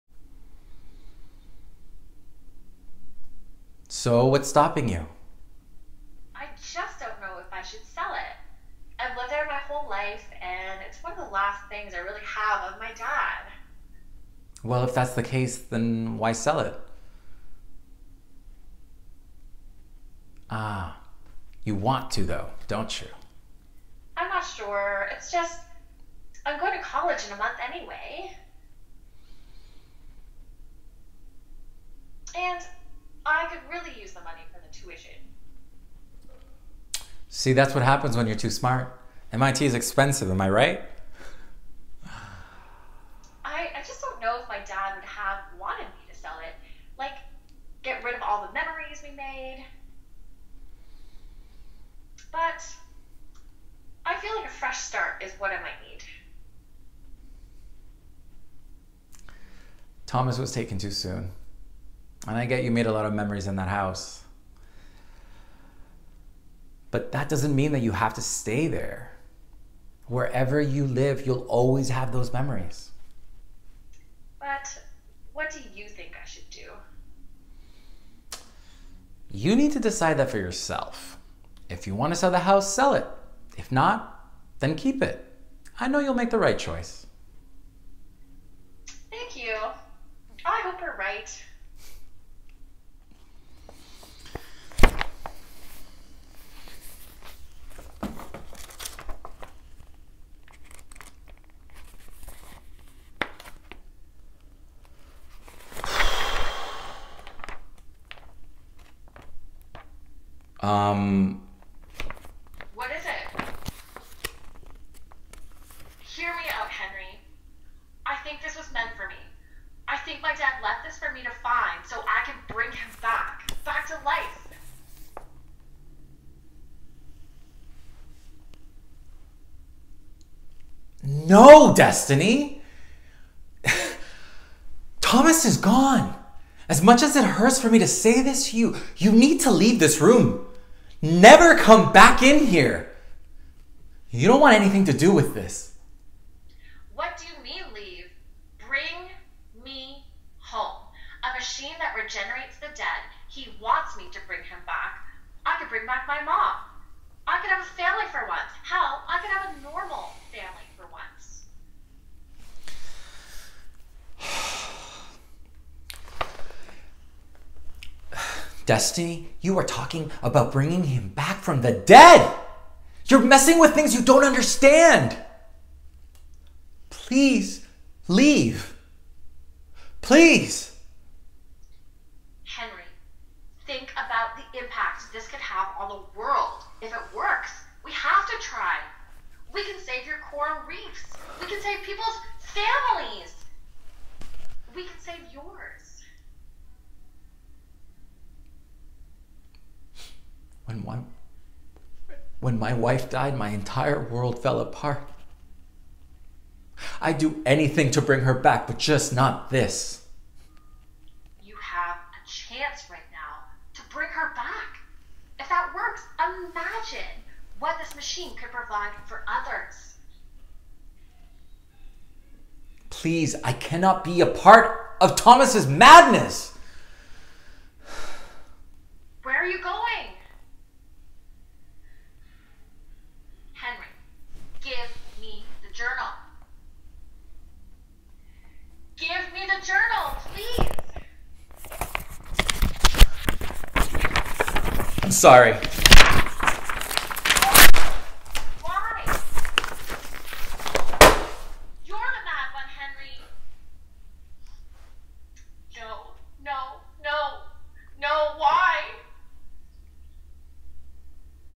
Soap Opera feels
I pulled out my days of our lives and general hospital feels for this young and the restless meets buffy the vampire slayer self tape. It's a long one but I sort of lose it by the end so its a fun watch. I also got a callback for this